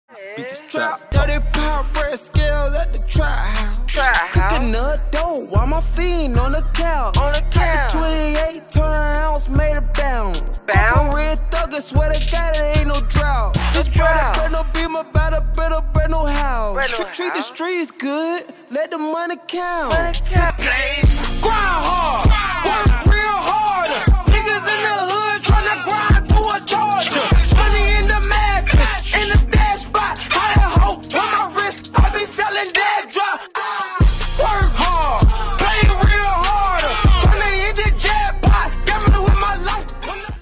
Little Hottle Birthday Weekend
Live Performances by Cool Amerika & SoLoyal T2G